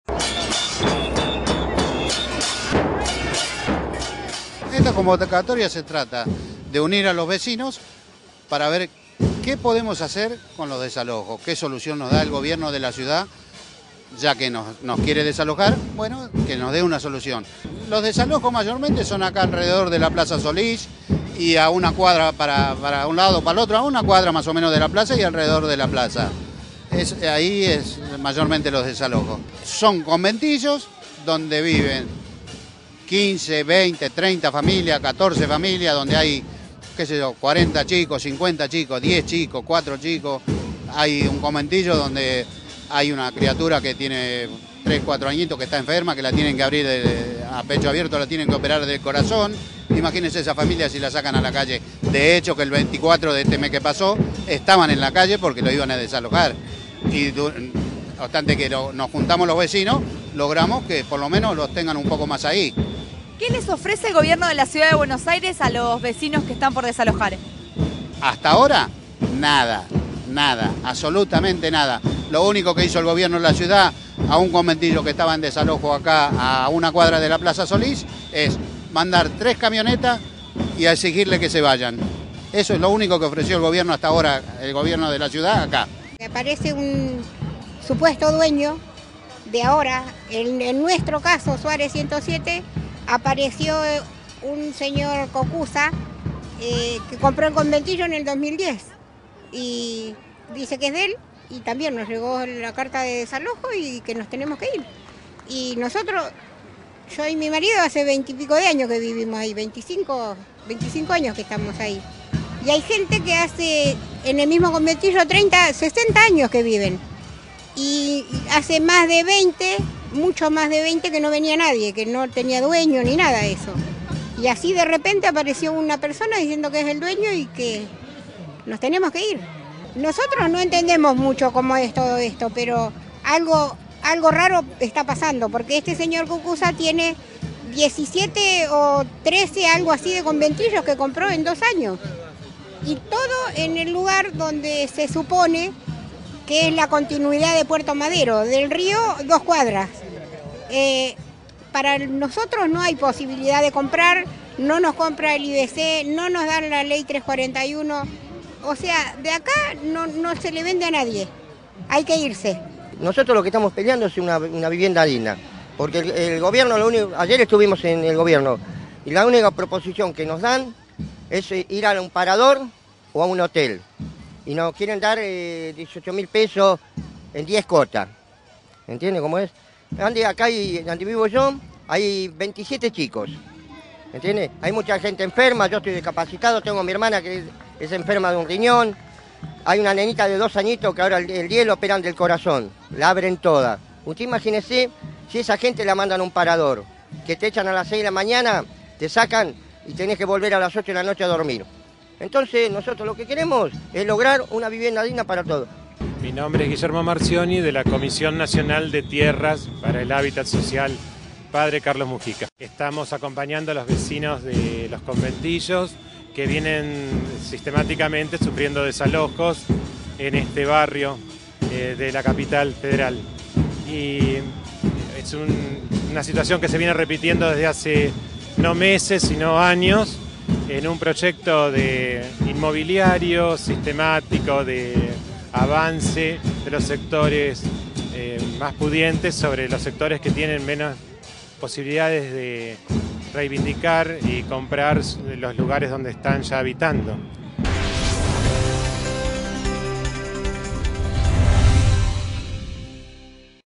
Denuncian desalojo de 17 conventillos de La Boca por negocio inmobiliario
La orden de desalojo para unas 300 familias que viven en 17 conventillos de La Boca, movilizó a vecinos de ese barrio porteño que realizaron una jornada con festival incluido bajo el lema "No a los desalojos" en la Plaza Solís.
En un comunicado de prensa los organizadores del festival en La Boca acusaron al macrismo de extender un circuito inmobiliario de gran escala "desalojando a las familias humildes, tirando abajo sus viviendas de toda la vida, para construir y apropiarse del terreno, para un proyecto excluyente para los habitantes históricos del barrio de La Boca y el sur de la Ciudad de Buenos Aires".
La "Usina", el "Distrito de las artes", la búsqueda de extender "Caminito" y seguir fomentando el turismo en desmedro de los sectores populares del barrio, son algunos de los hechos por los que los vecinos consideran que buscan desalojarlos de sus viviendas.
La convocatoria tuvo como objetivo unirse "para buscar y reclamar soluciones al gobierno de la Ciudad ante el posible desalojo", expresaron los vecinos.